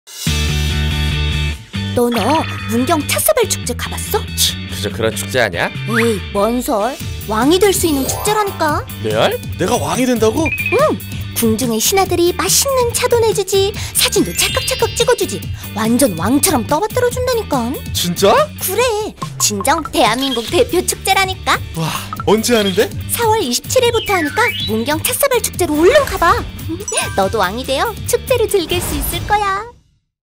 2019 문경찻사발축제 스팟광고
2019 문경찻사발축제 스팟광고영상

문화체육관광부선정 대한민국 대표축제

2019 문경찻사발축제

쉬고 담고 거닐다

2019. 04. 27.(토) ~ 05. 06.(월) 10일간 문경새재오픈세트장